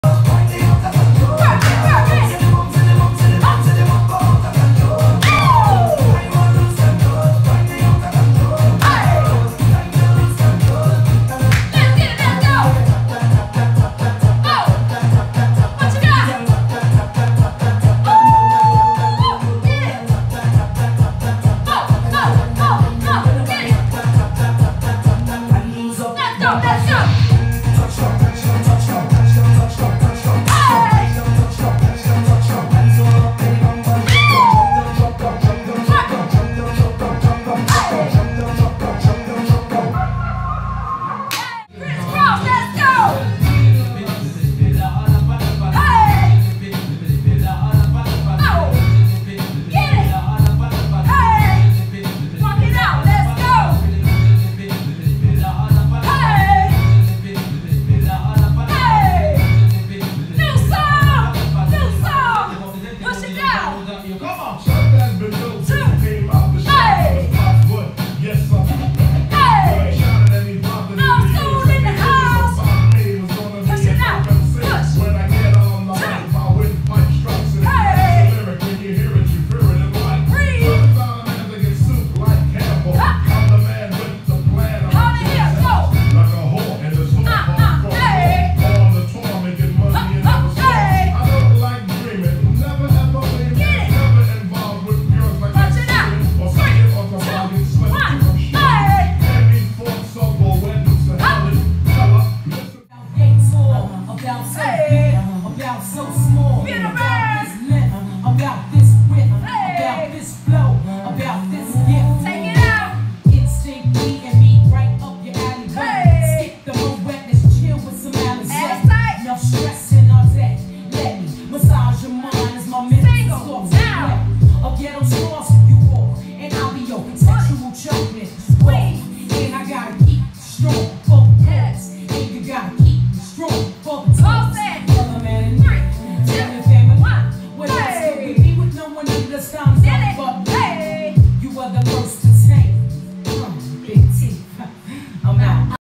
STAY LIT NEW CHOREO DROPS
🔸🔸CATCH THE REPLAY🔄 🔸🔸
We had an absolute BALL in our Monday Night STAY LIT Virtual class!!! 🎉🔥💃🏽💪🏾🤗Here’s a few of the NEW CHOREO DROPS we enjoyed!! You can catch the LIVE RECORDED REPLAY for only $5, which will give you 24 hour access to the video. LET’S GET ITTTT!!!